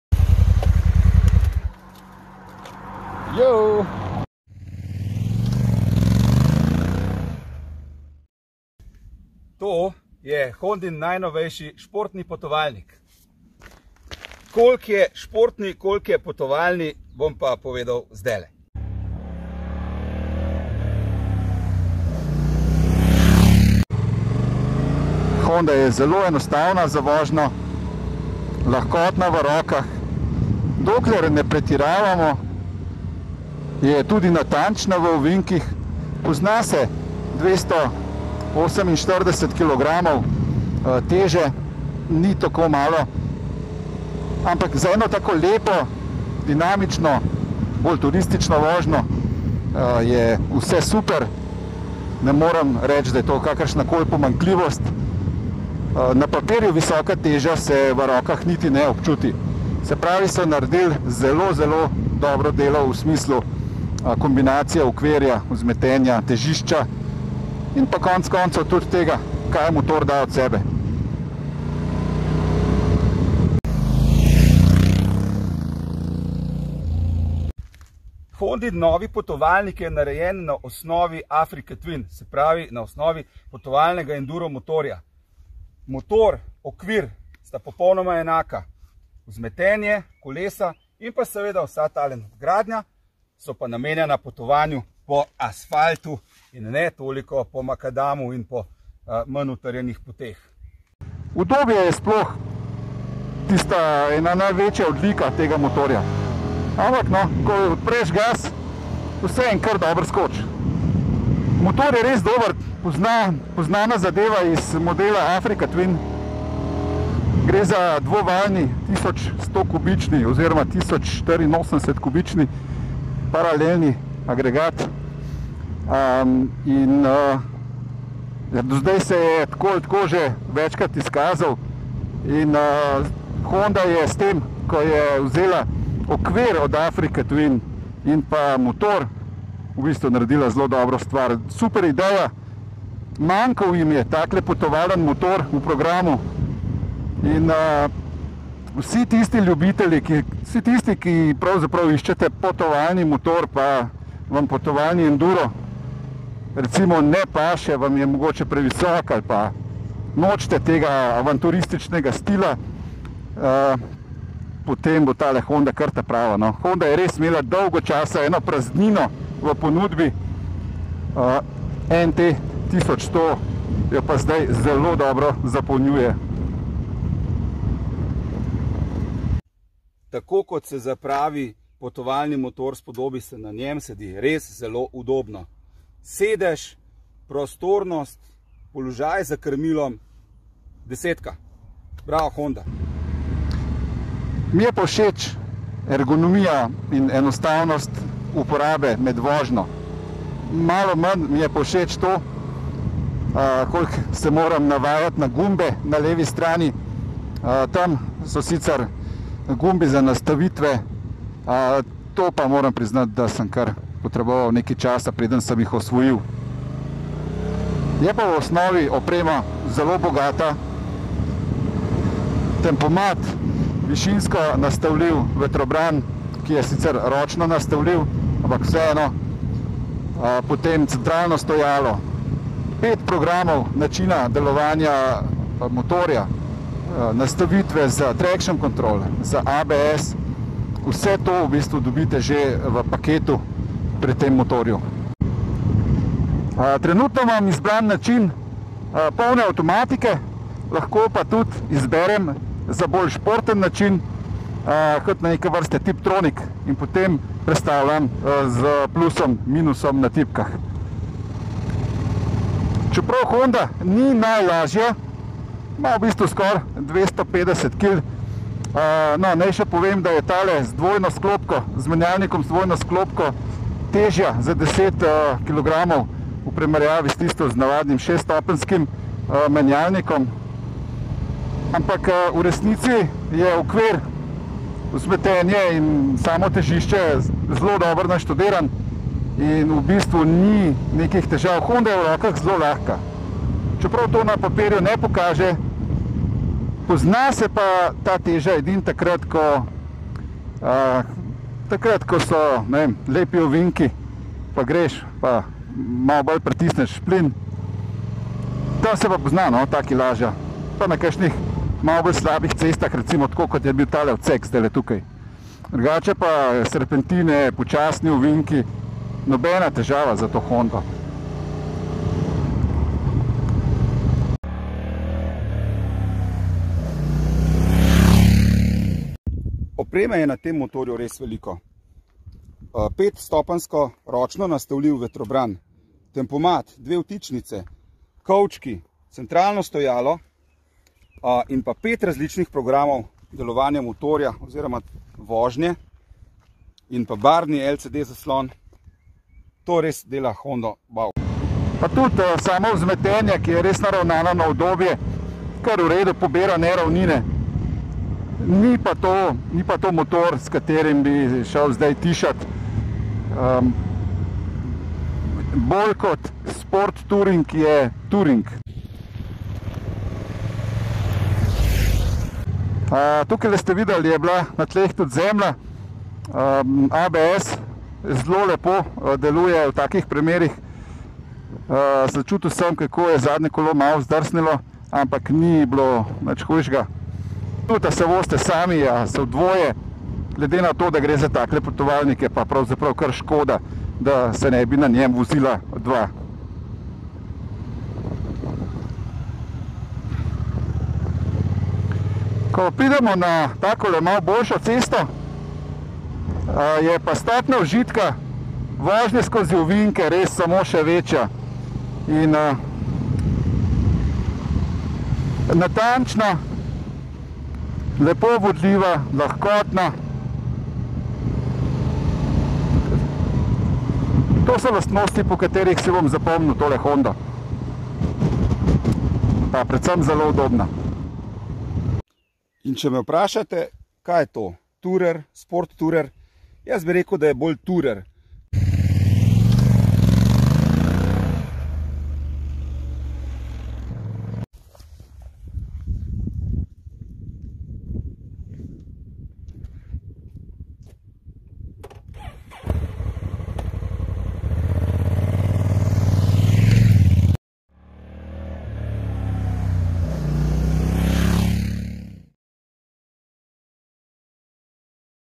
Honda NT1100 DCT - Test Motoavantura.si
TEST - HONDA NT1100 DCT
Tako se pelje najnovejša športno potovalna Honda...
Cena za osnovni model znaša 14.490, takšna z dvojno sklopko - torej avtomatik pa 1100 evrov več. Motor je zelo dober potovalnik, udoben, lahkoten v vožnji in zanesljiv. Predvsem pa ima že v osnovi ogromno opreme...vključno s stranskima kovčkoma. Poraba je zmerna in ob dinamični vožnji znaša 4.5 l na 100 km. Ko govorimo o vožnji v dvoje in razmerju med ceno in tem kaj dobiš je tole sedaj eden najboljših motorjev za udobna potovanja.